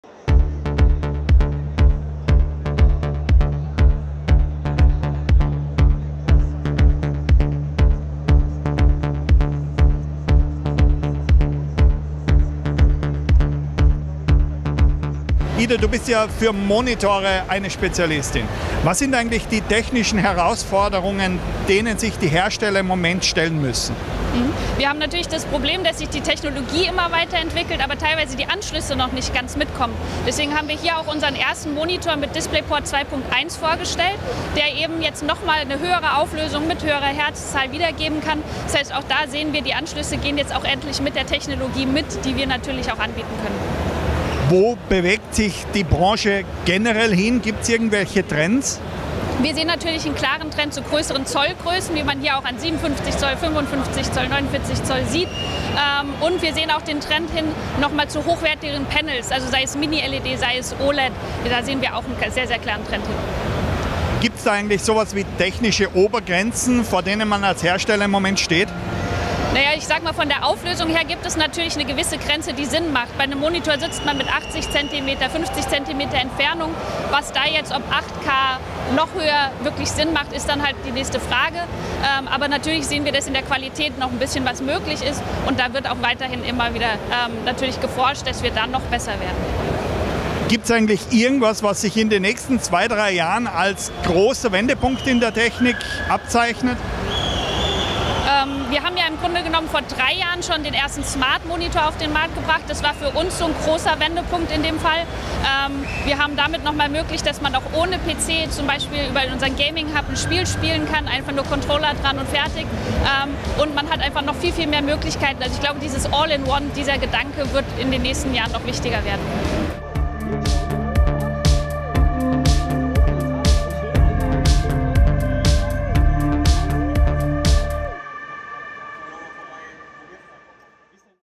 Gamescom Interview - Trends bei Monitore
Wir haben uns auf der Gamescom nach den neuesten Trends bei Gaming-Monitoren umgeschaut und war dafür bei der Firma Samsung

𝓻𝓮𝓮𝓲𝓷𝓼.𝓽𝓿